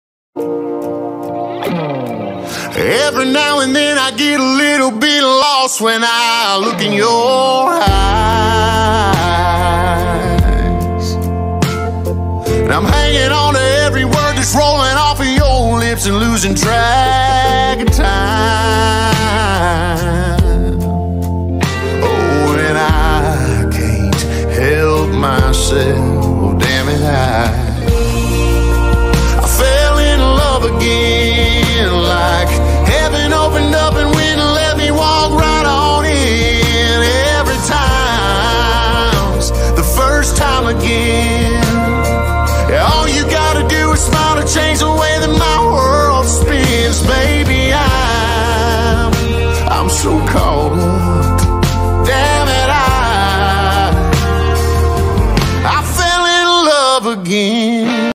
Kat Cunning Video I made for Tik Tok using the orignal sound by Kyle Coulahan
Wait for the pic at :40 when Kyle sings the first time again - that is the first time I ever saw Kat in the Netflix Series Trinkets - They play Sabine